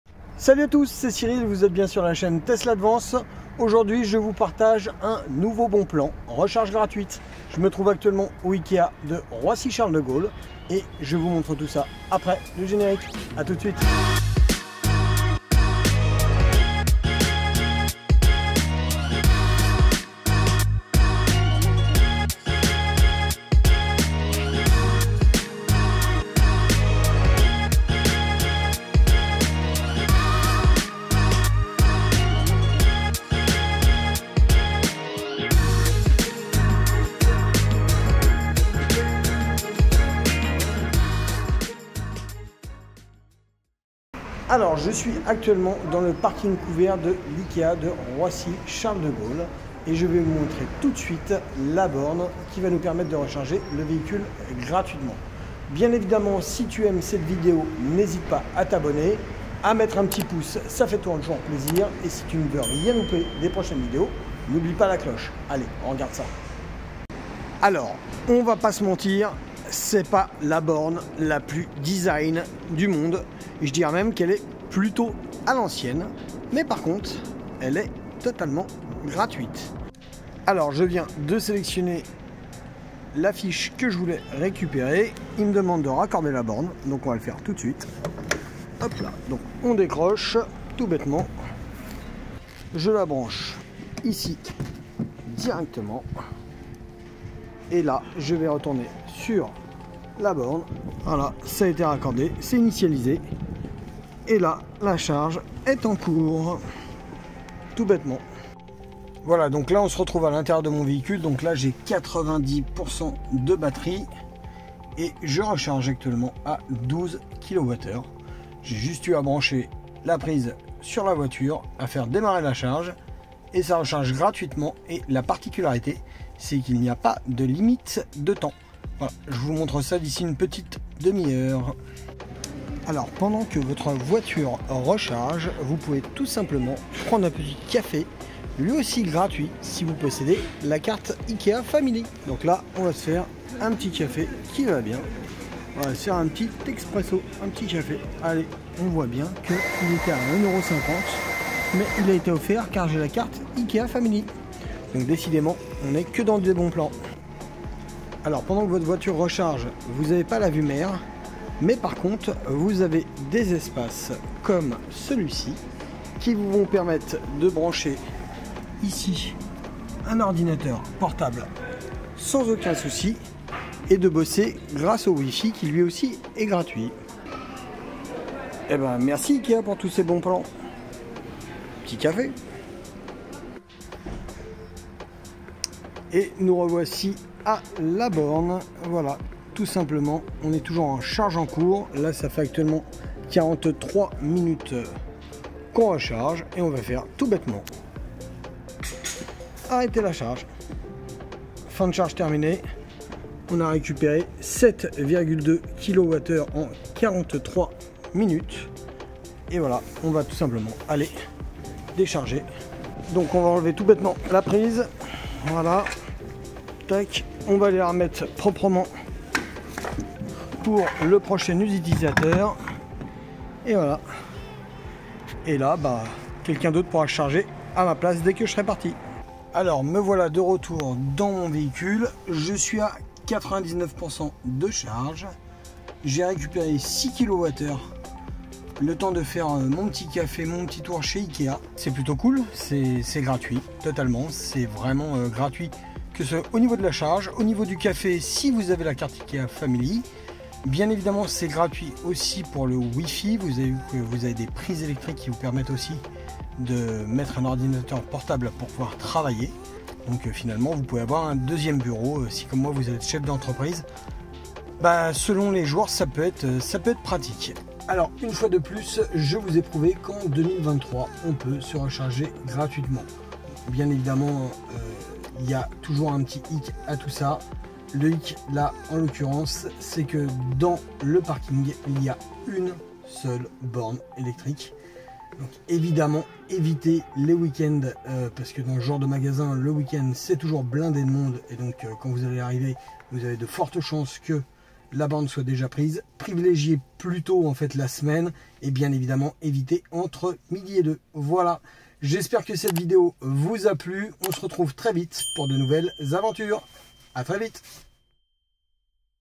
BON PLAN DU JOUR ! RECHARGE GRATUITE CHEZ IKEA ET BIEN PLUS ENCORE :)
Aujourd'hui je teste la recharge gratuite chez Ikea mais je vous fais aussi découvrir des petites astuces sympa le temps de la recharge !!!